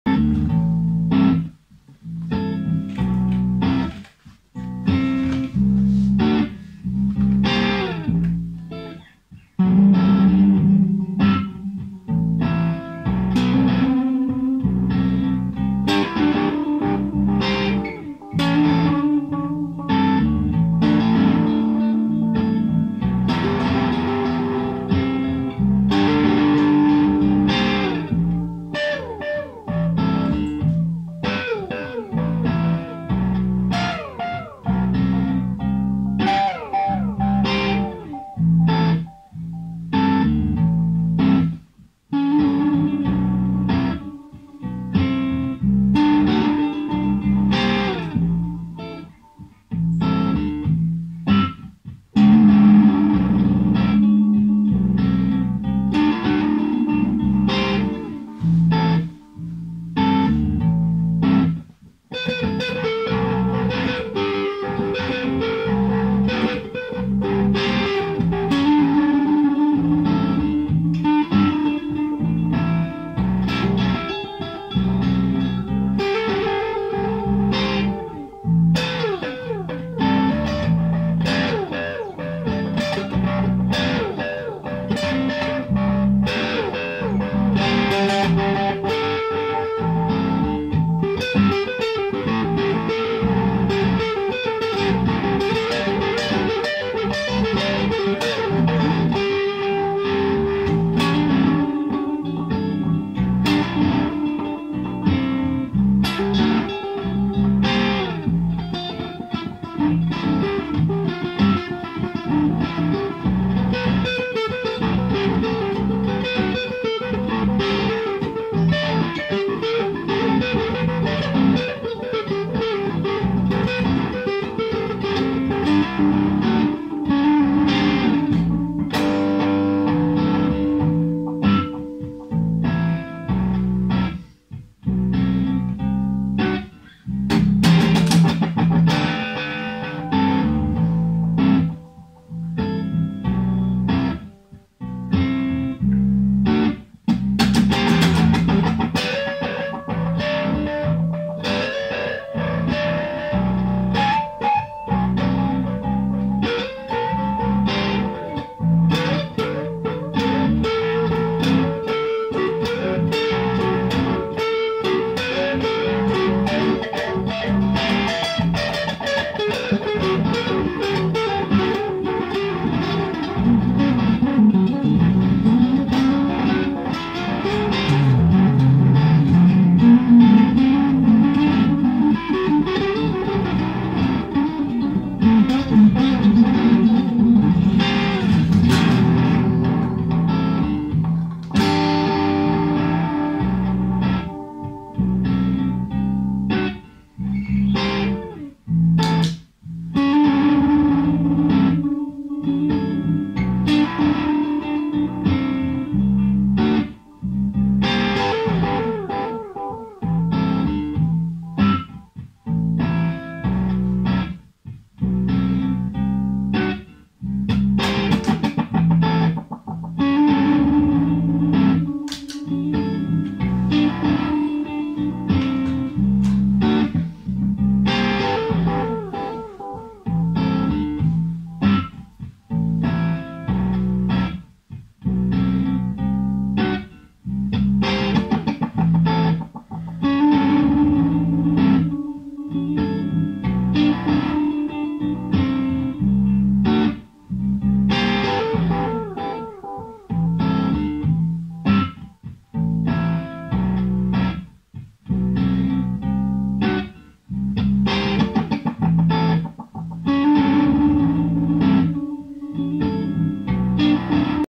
𝙏𝙖𝙡𝙠𝙞𝙣𝙜 𝙞𝙣 𝙩𝙝𝙚 𝙨𝙥𝙖𝙘𝙚 / 𝘿𝙖𝙣𝙘𝙞𝙣𝙜 𝙞𝙣 𝙩𝙝𝙚 𝙨𝙥𝙖𝙘𝙚
Come risuona? Fammi sapere qui sotto con una risposta.

Scrivi un commento 🙂
Lascia un pollice sù 👍
Segui il canale 📺
grazie! 🙃

*strumentazione usata*
DelayLab
OC3

1995 Fender Stratocaster modded
EHX 15Watt Howitzer
12" cabinet handmade

Mi chiamo Fabrizio Lembo Gray e sono un chitarrista e un cantante. Ciao 🖐️